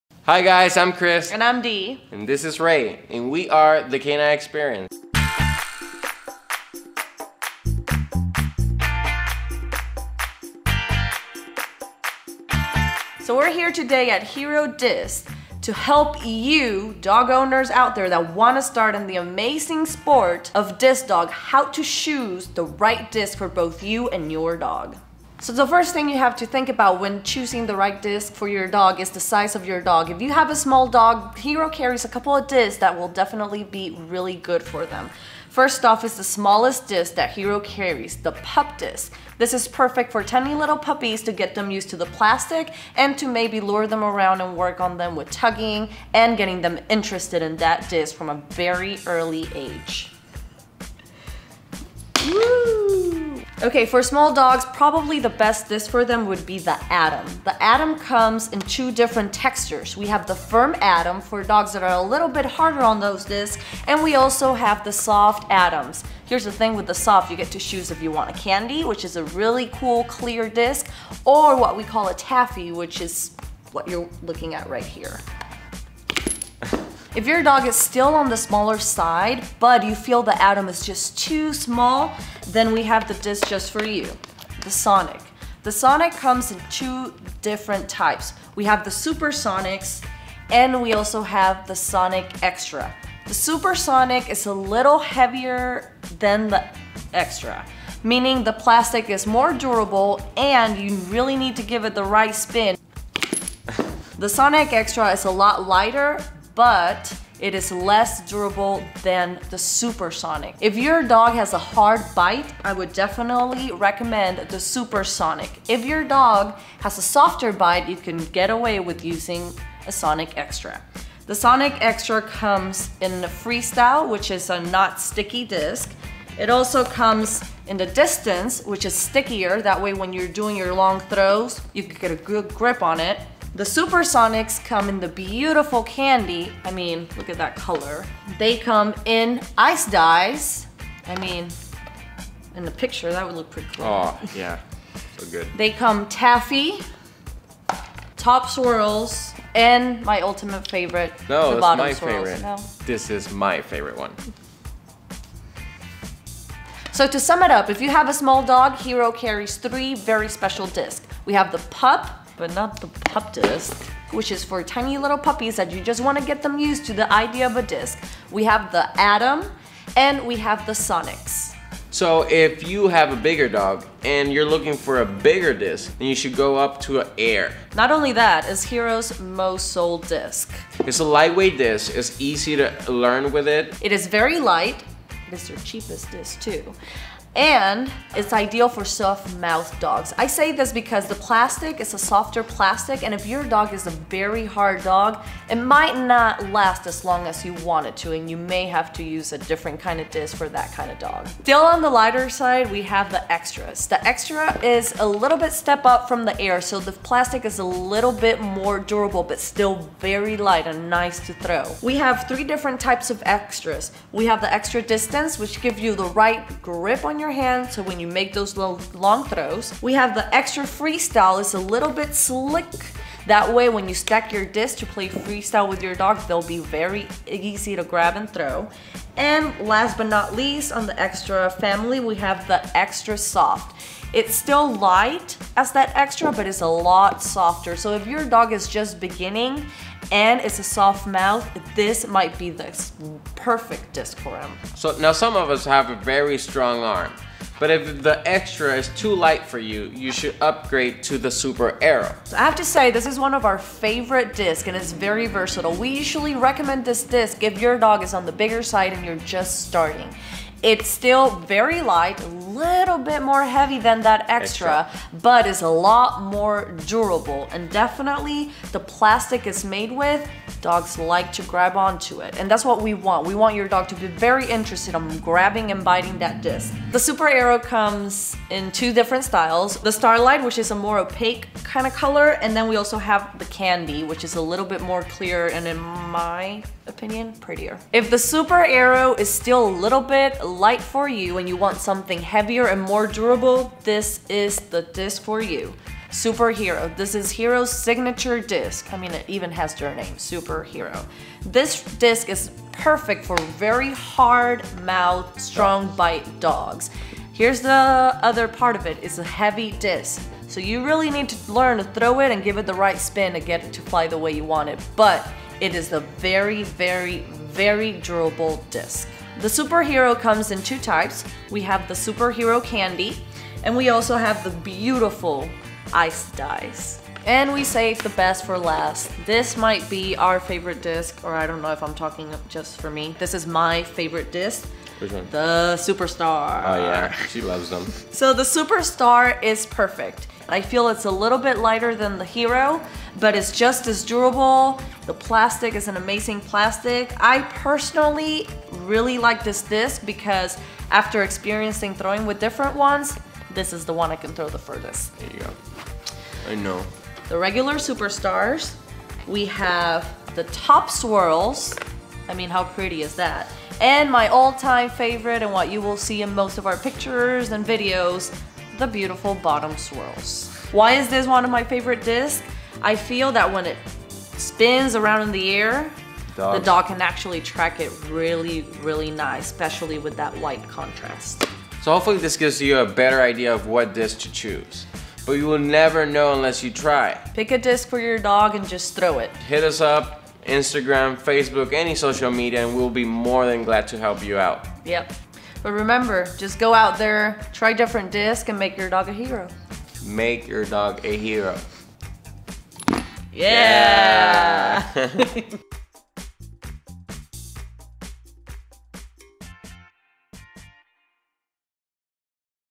Choosing Your Hero Disc
Chris and D of The Canine Experience help you get started in disc dogging and choosing the best Hero Disc for you and your dog.